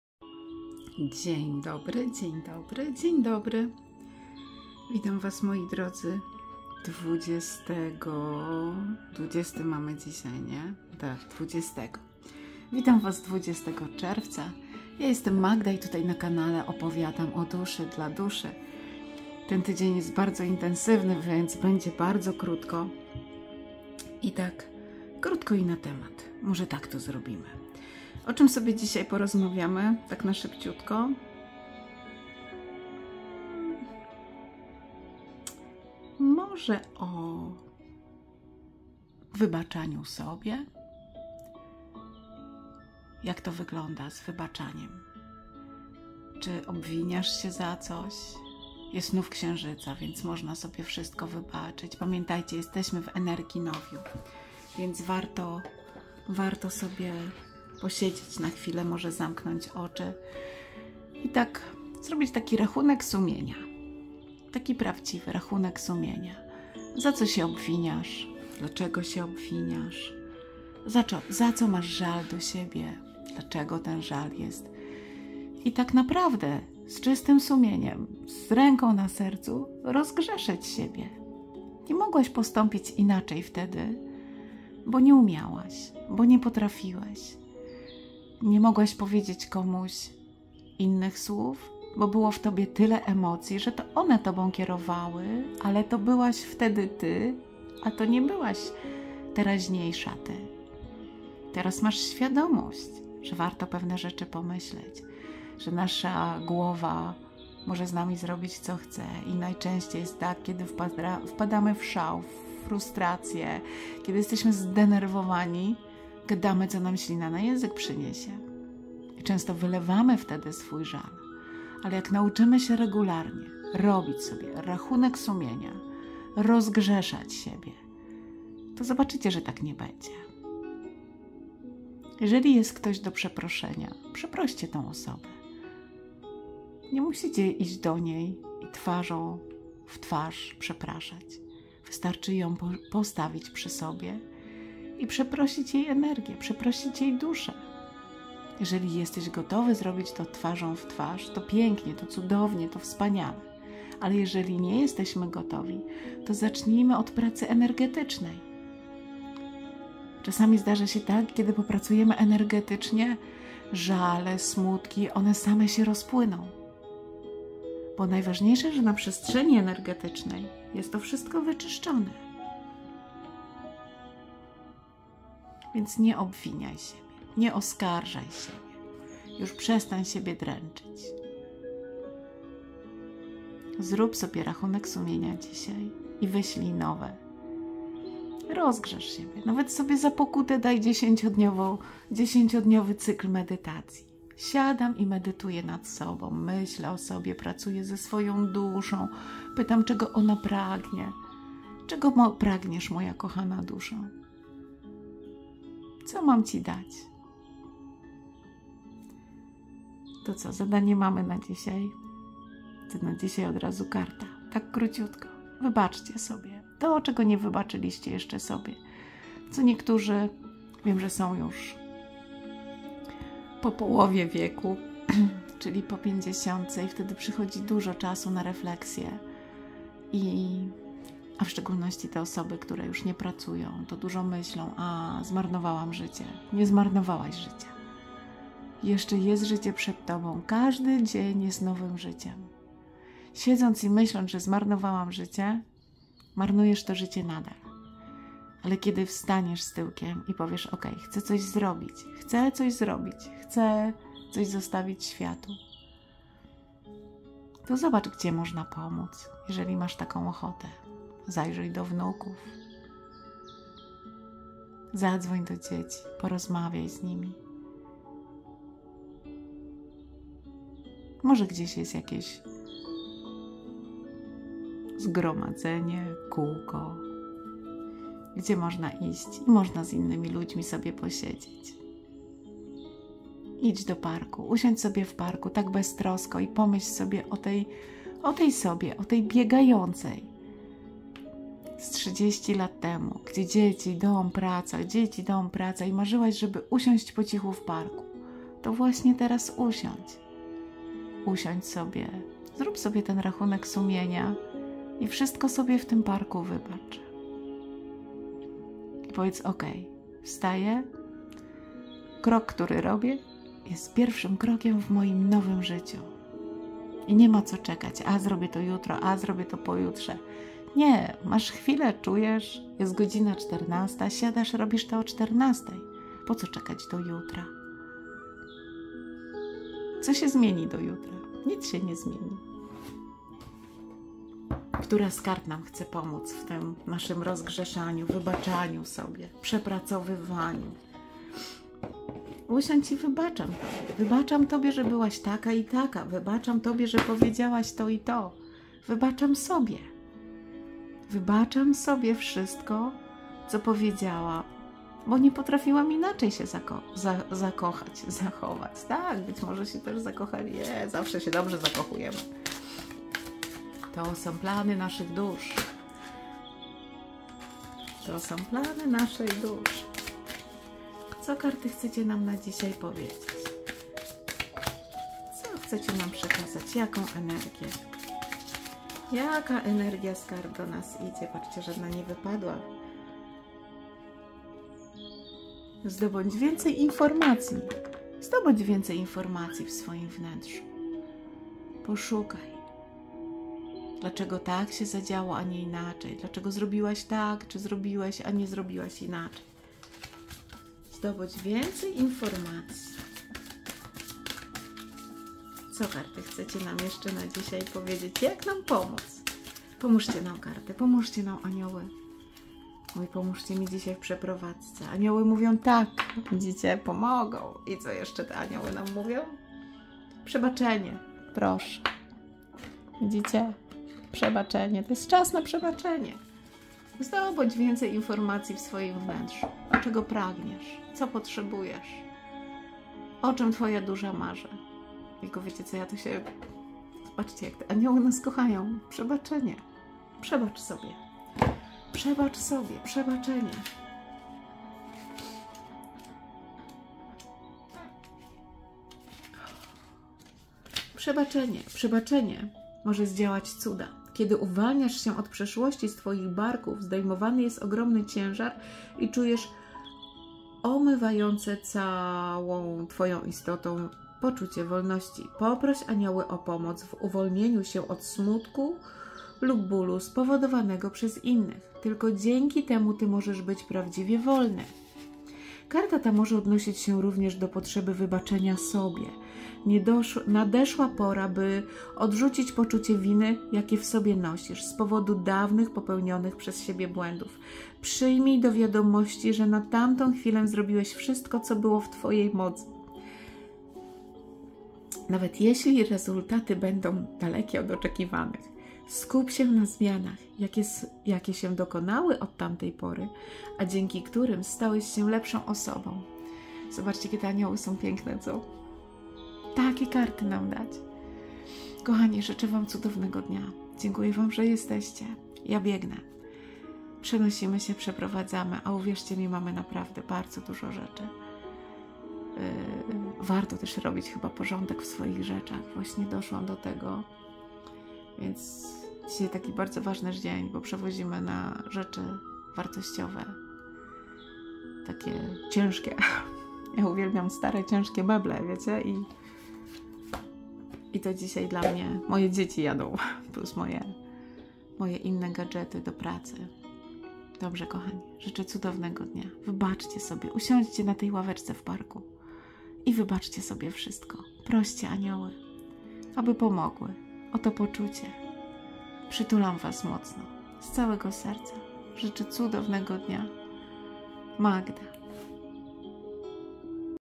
20.06.2023 Przesłanie z kart "PRZEBACZENIE"
Nadeszła pora,  by odrzucić poczucie winy, jakie w sobie nosisz z powodu dawnych popełnionych przez Ciebie błędów. Przyjmij do wiadomości, że na tamtą chwilę zrobiłeś wszystko, co było w Twojej mocy, nawet jeśli rezultaty były dalekie od oczekiwań. Poproś Anioły o uwolnienie się od smutku lub bólu. Tylko dzięki temu możesz być prawdziwie wolny.